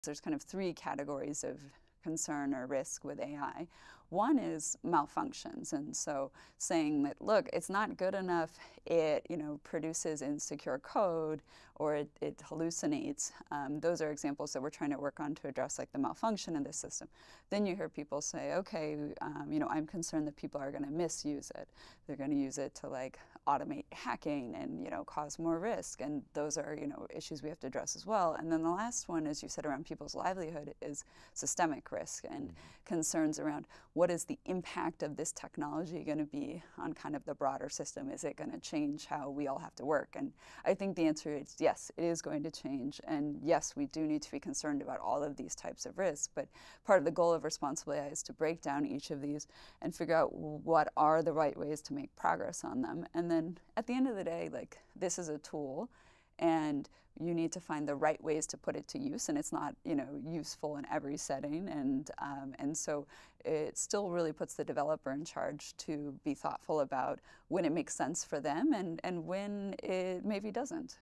THE 3 MAIN AI RISKS
Dr. Sarah Bird breaks down the main AI risks into three categories:
1. MALFUNCTIONS: such as producing insecure code or hallucinations
2. MISUSE: things that cause more risk like automating hacking
3. SYSTEMIC: the impact of these technologies on the broader systems, our careers, and livelihood

the full interview: syntax.fm/941

Hit us up on Socials!